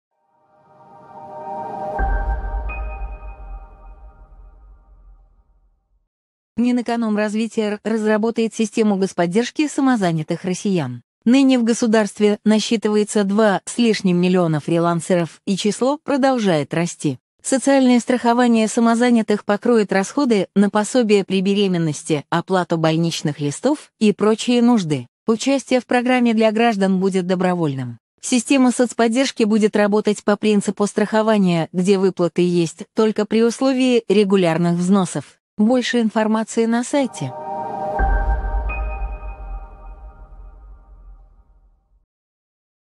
Самозанятые получат возможность оформить соцстрахование
В Минэкономразвития предложили разработать меры социальной поддержки самозанятых граждан. Подробнее